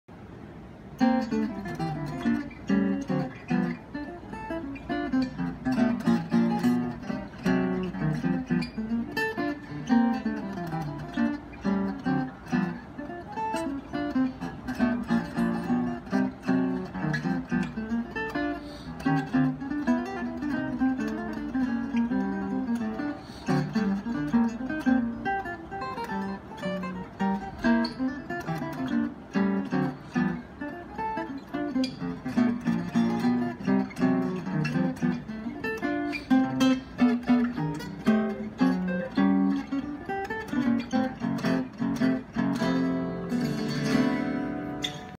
Pasquale Grasso playing an unfinished archtop Valle Guitar.
Pasquale Grasso trying my first archtop guitar built at Wyatt Wilkie's shop in Vancouver Island on his 10 day master class!